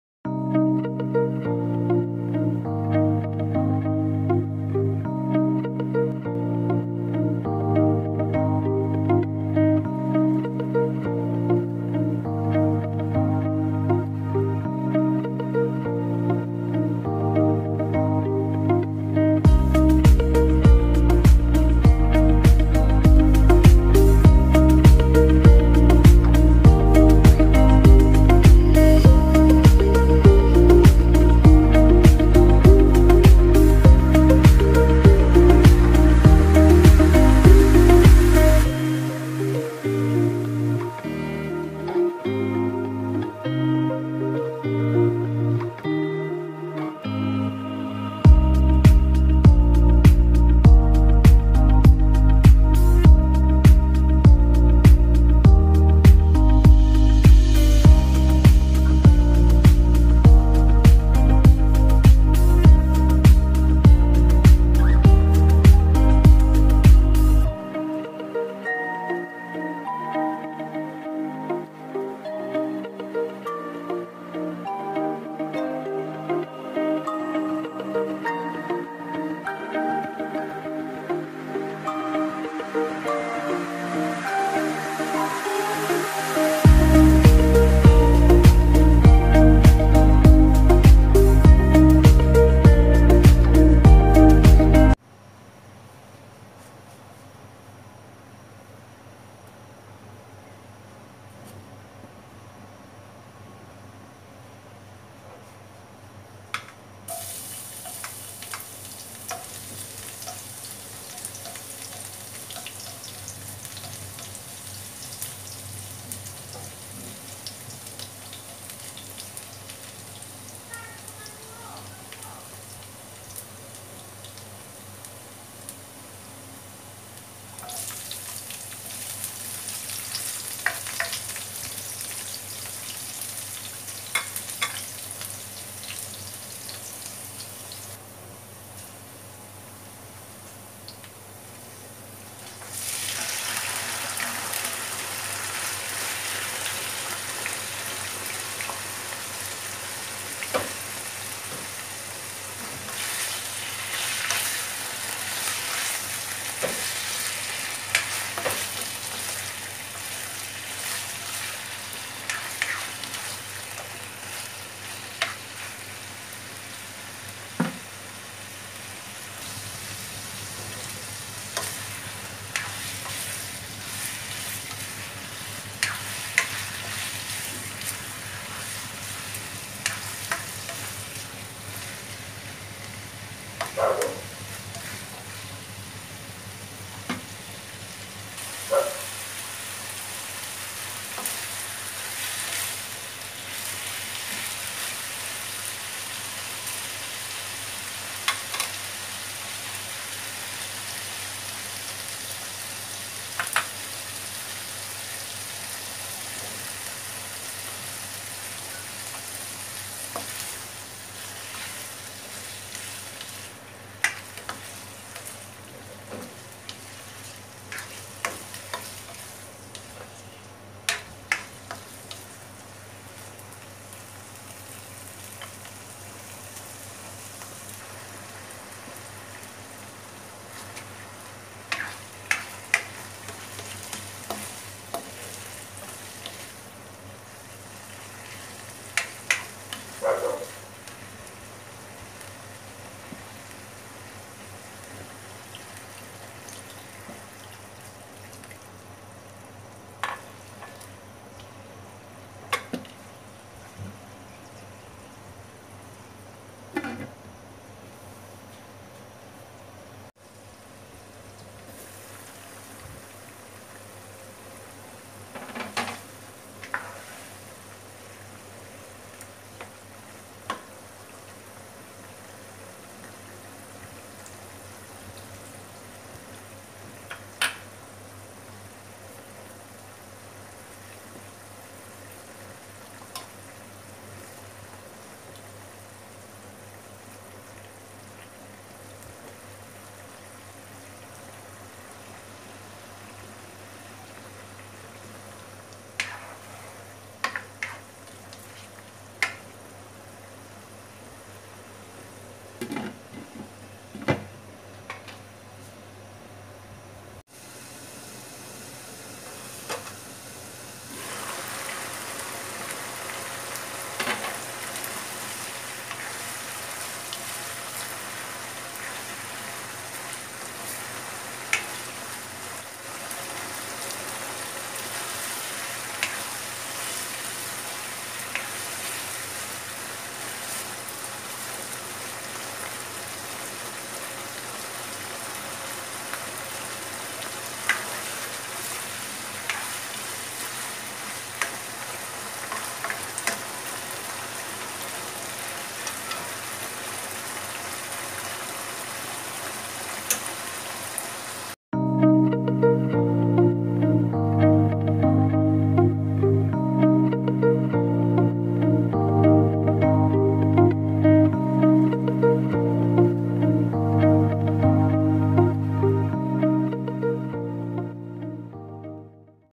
Adobong Baboy/Marinated Pork Adobo|Sharvie Cooks
Hello! I'm Sharvie. In this video i will be showing you how i make my own version of pork adobo .I hope you will enjoy watching this video.